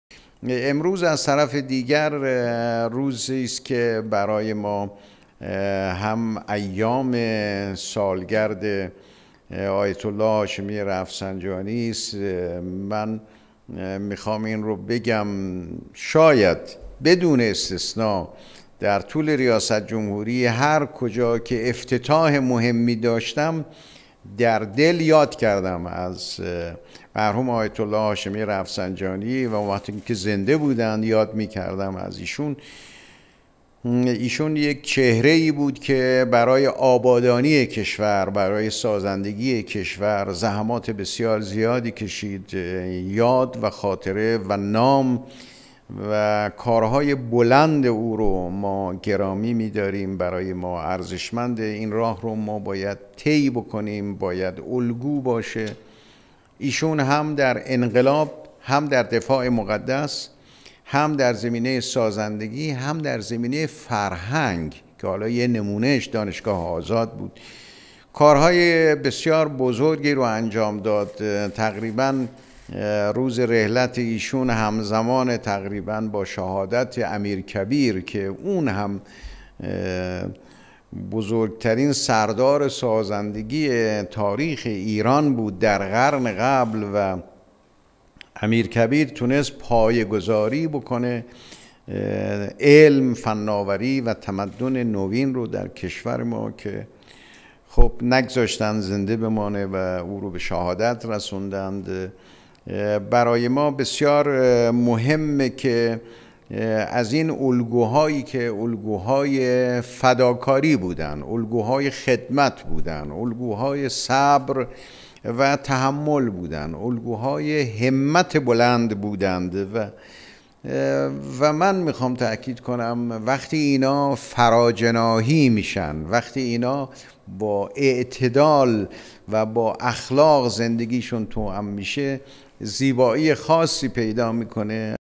مرحوم آیت‌الله هاشمی رفسنجانی برای آبادانی و سازندگی کشور زحمات بسیار زیادی کشید
مرحوم آیت‌الله هاشمی رفسنجانی برای آبادانی و سازندگی کشور زحمات بسیار زیادی کشید/ برای ما بسیار مهم است که از امیرکبیر و آیت الله هاشمی رفسنجانی که الگوهای فداکاری و خدمت، صبر و تحمل و الگوهای همت بلند بودند، تجلیل کنیم